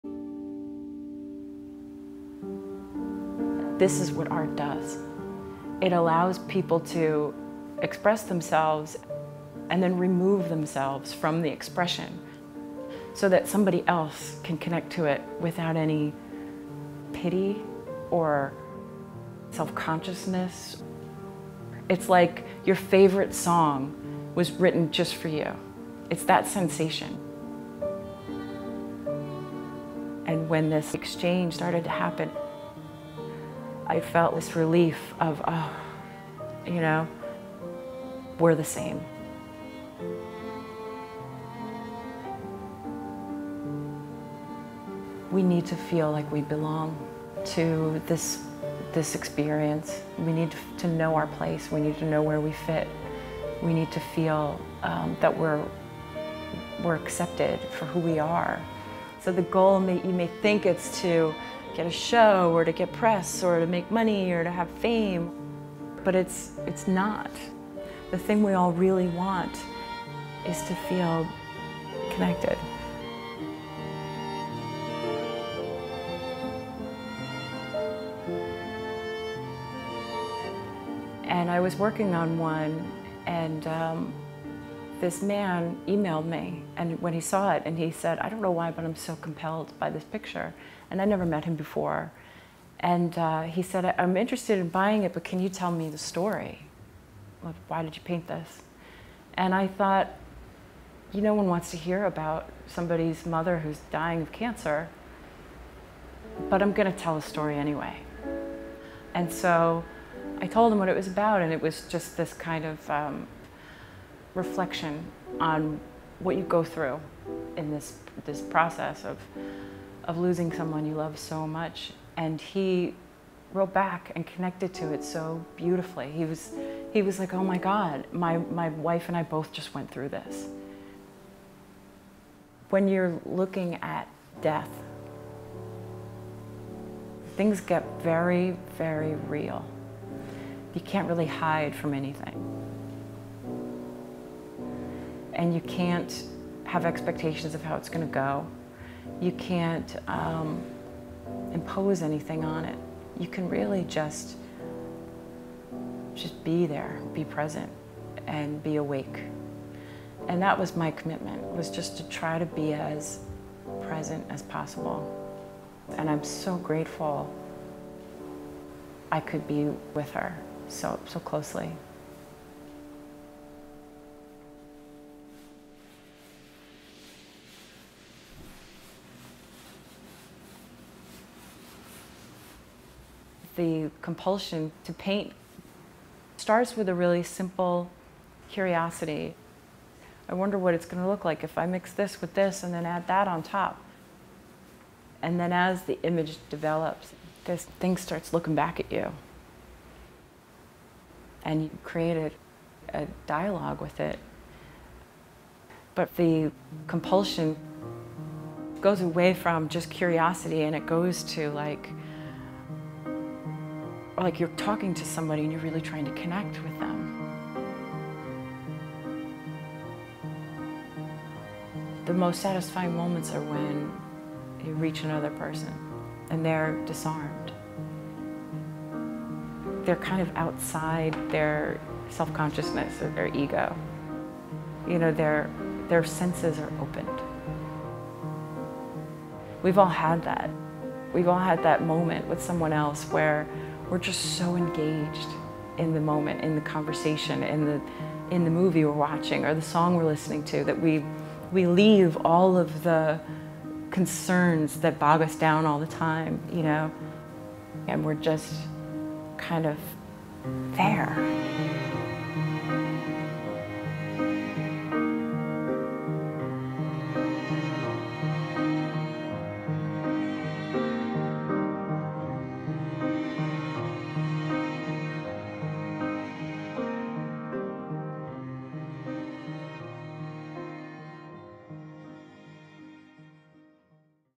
Connection // Alyssa Monks
Making Art New York, Alyssa Monks, Brooklyn, New York
"We need to feel like we belong to this experience, we need to know our place, we need to know where we fit. We need to feel that we're accepted for who we are. So the goal you may think is to, get a show, get press or to make money or to have fame. But it's not, the thing we all really want is to feel connected."
You can find out more about Alyssa here: alyssamonks.com
For more information about Brass Brothers Films go to brassbrothersfilms.com.